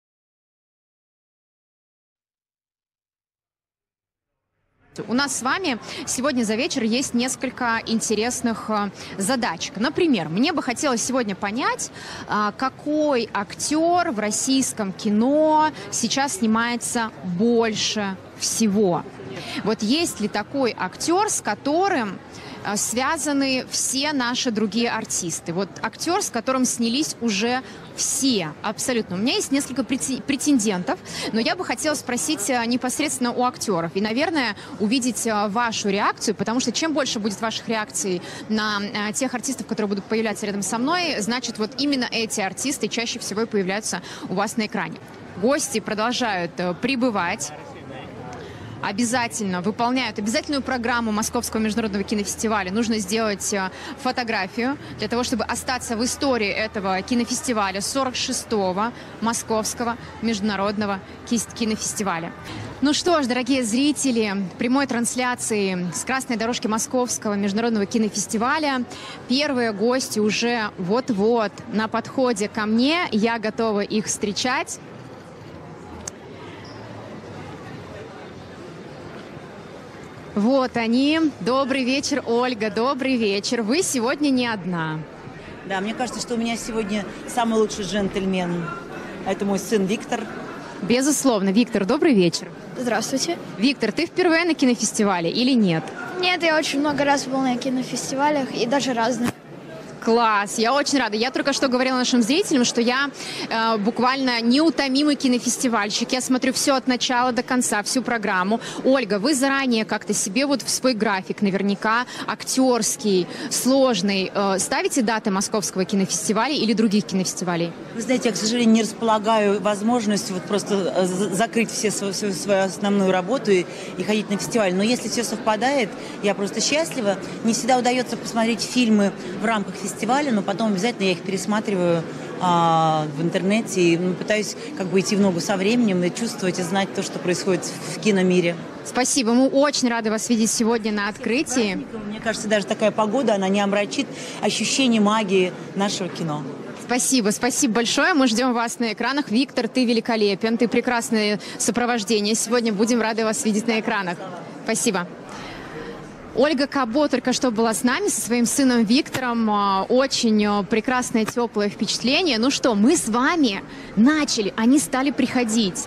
Ольга Кабо на 46 ММКФ
Мне бы очень хотелось опубликовать всю прямую трансляцию открытия 46 ММКФ, так как RUTUBE это делает впервые. И на мой взгляд посмотреть это стоит. Поэтому думаю...
А Ваше мнение?
Ваша Ольга